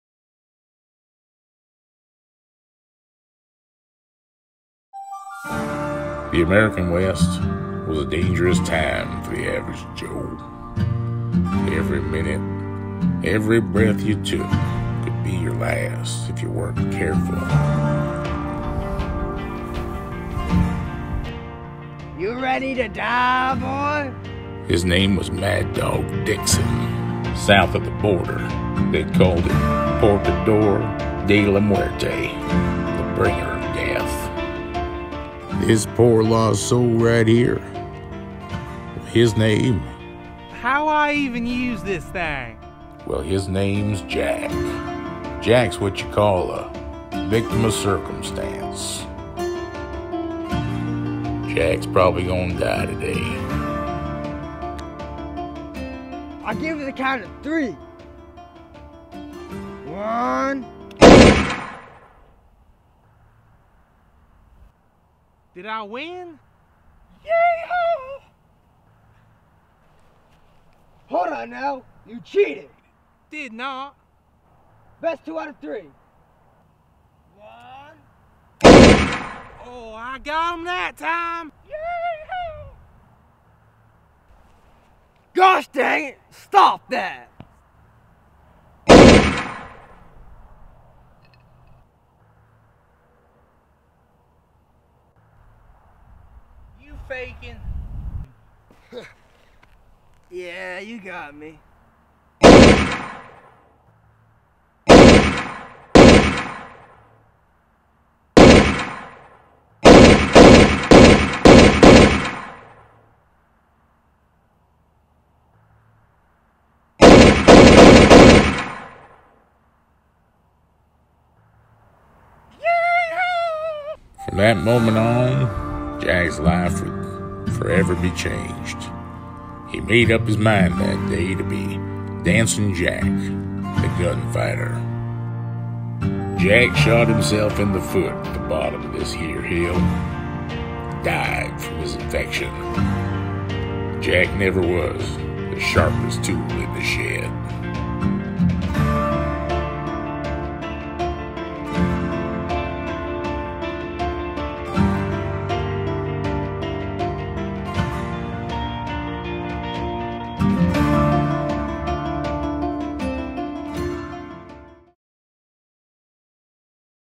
The Birth of a Gunfighter
This film was made for the Tap into Film 72 Hour Film Festival.  We made this film at home while social distancing with just what we had in the house and some creativity.  The criteria we had to incorporate into the film was 1.) Dancing 2.) Some multi-cultural reference 3.) An unlikely hero and 4.) Our assigned genre:  Western.

We hope you'll enjoy and vote for our film as a crowd favorite.  To vote follow this link:

A confirmation email of your vote will be sent to your email.  Go to your email and click the link provided to register your vote.

We know that's a few steps but we greatly appreciate the support!